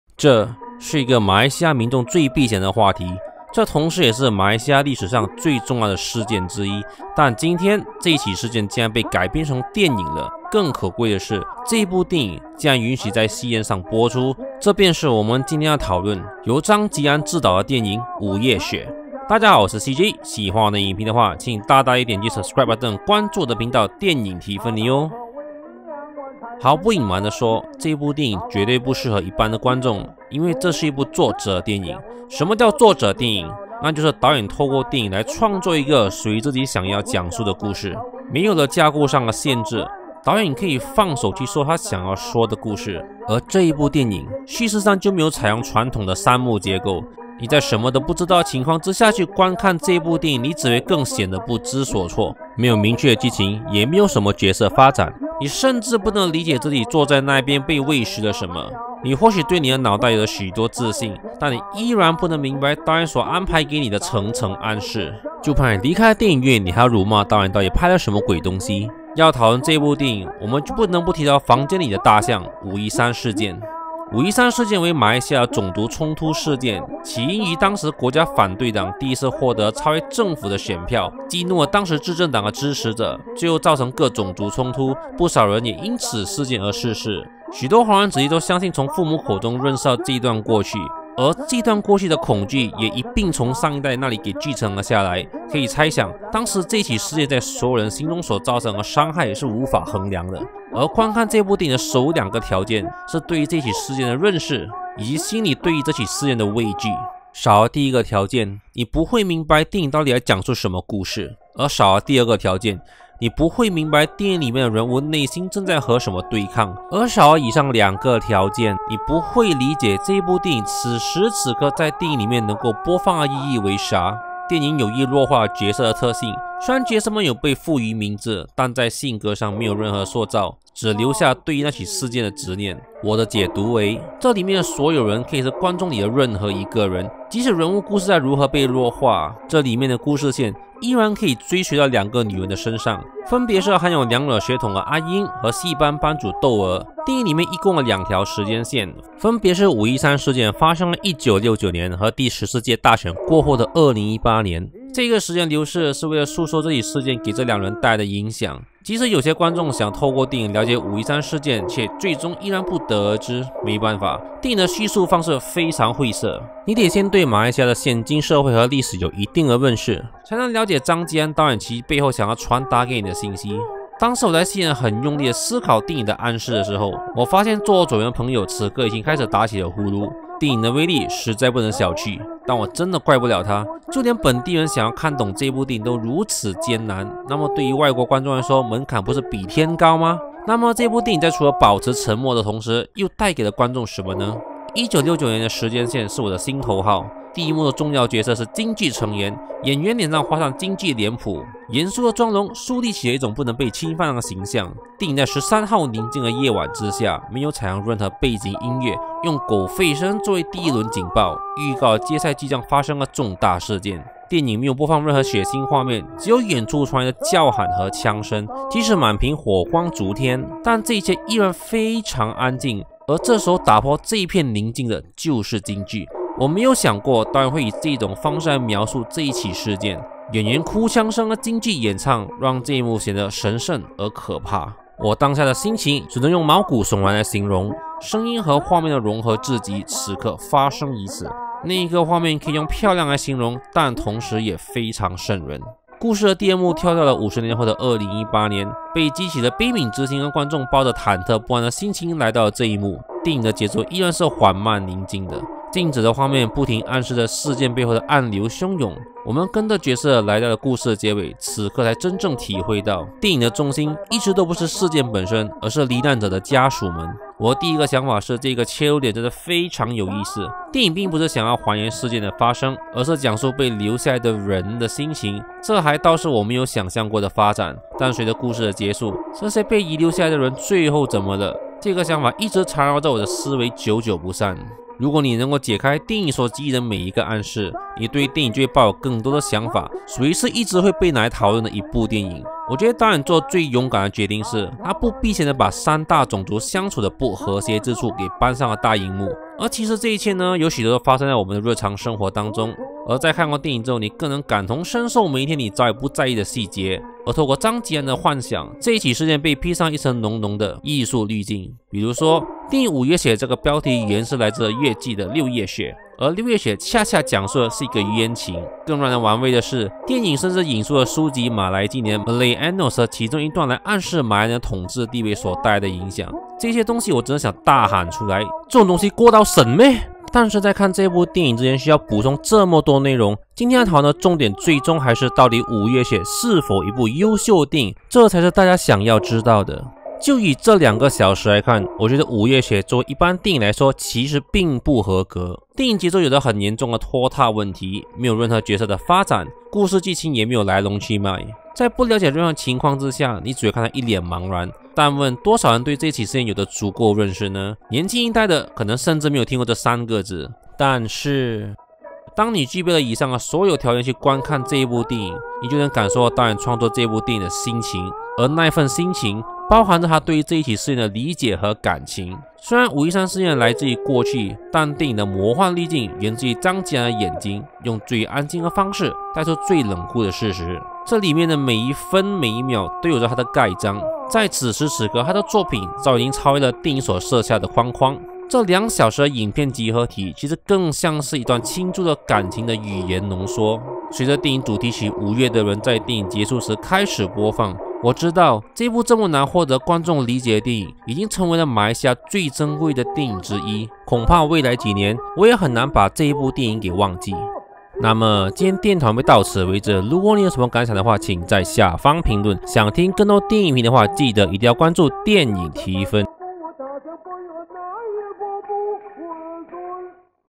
千言万语所浓缩而成的两个小时，《五月雪》已经不能称为一部电影
《五月雪》电影点评

喜欢看电影的人，喜欢看点评的人，还有喜欢了解电影更多来龙去脉的朋友都可以关注我们了解最新的电影！！